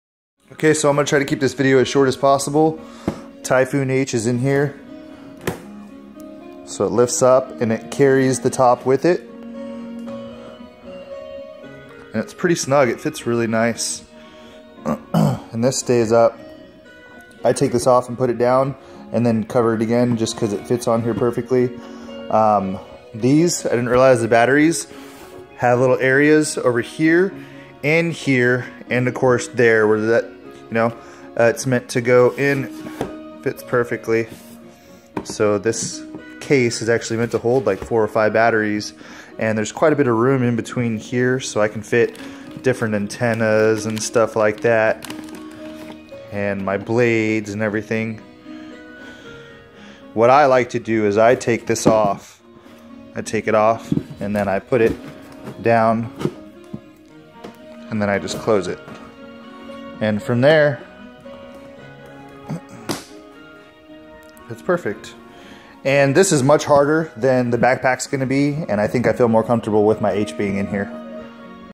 Yuneec Typhoon H 4k finished fitting foam case to q500 hard case amazing
Just finished feeding the foam case to the q500 hard shell case it works amazing it's much stronger than the backpack is going to be and honestly carrying around a backpack just makes me prone to throwing it around. I feel a lot more comfortable with it in this case